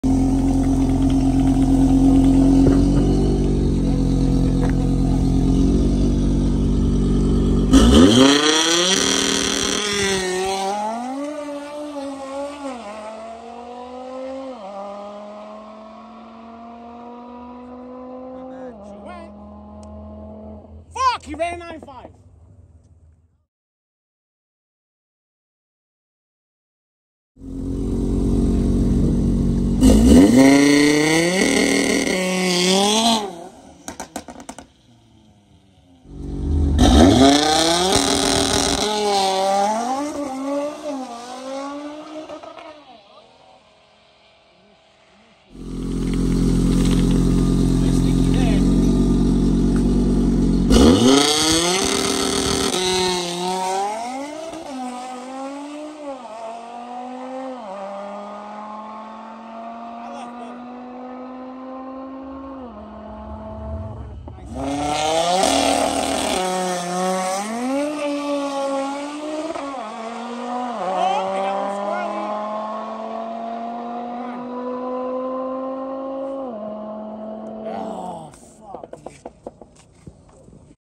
915 whp RS3 IMS850
Iroz IMS850 turbo kit, built engine, built trans, back seat delete, DS1 software. Best ET 9.5, best MPH 149. Bradenton Motorsports Park. Built by Miguel at RSP in Bradenton Florida.